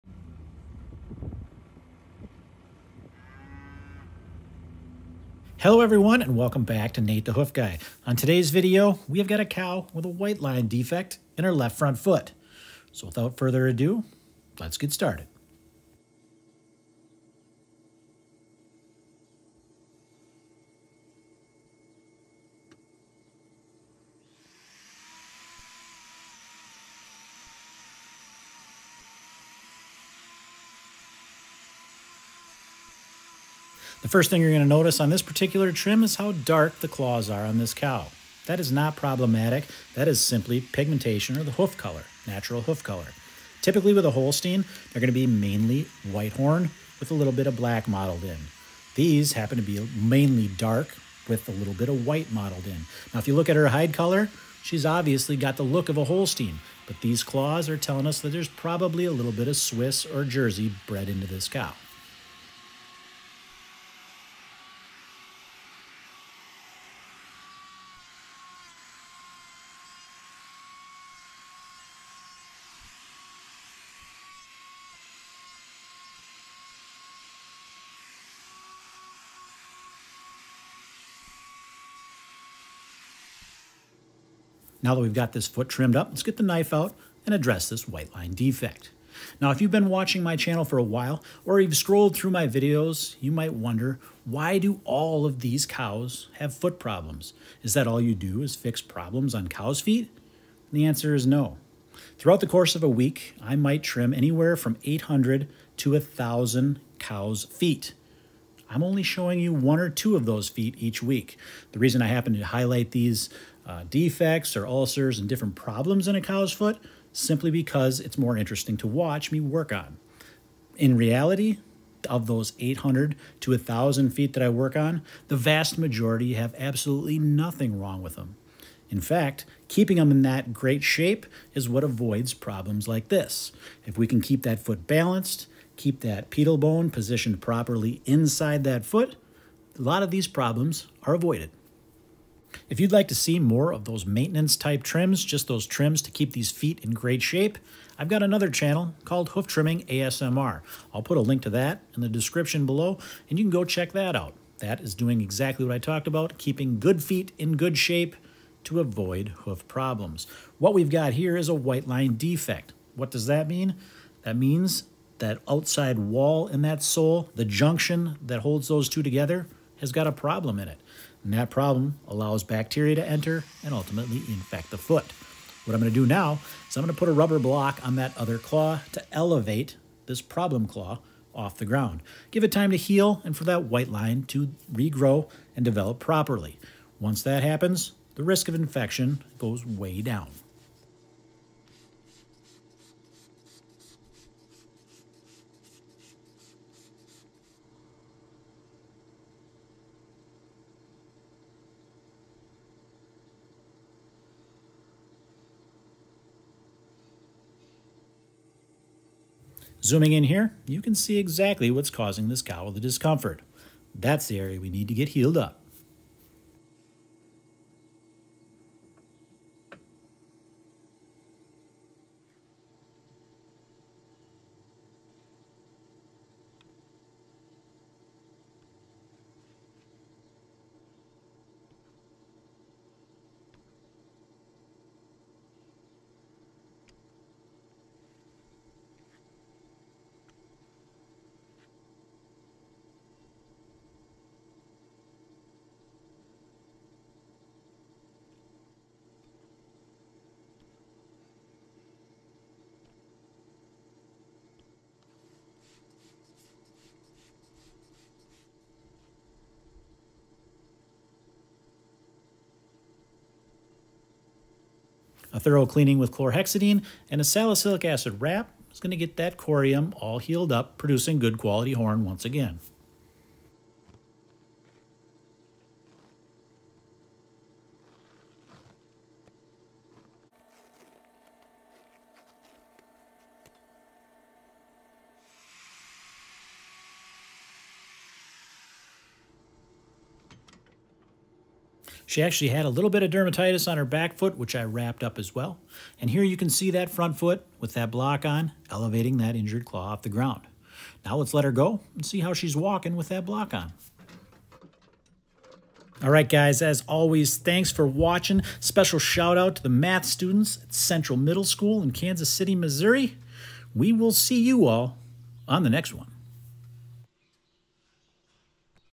Why do SO MANY dairy cows have HOOF PROBLEMS like this???
Today’s video we treat a cow with a WL defect and discuss what the majority of hoof trimming work entails.

Hoof Trimming ASMR